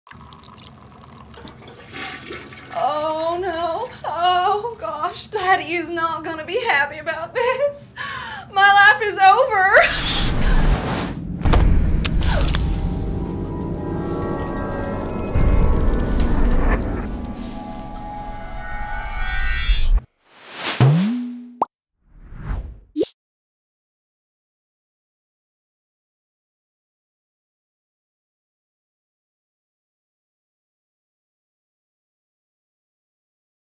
Pregnancy Scare (A 20 Second Horror) TRIGGER WARNING!
Pregnancy Scare is our submission to the "SCARE US IN 20 SECONDS" Fx Home contest. Enjoy!

[WARNING] Trigger warnings related to:  Pregnancy, stabbing, blood/gore

Starring: Marie Queenan and Jessey Nelson

Written by: Jessey Nelson

Directed by: Jessey and Cody Nelson

Cinematography: Cody Nelson

Edited by: Cody Nelson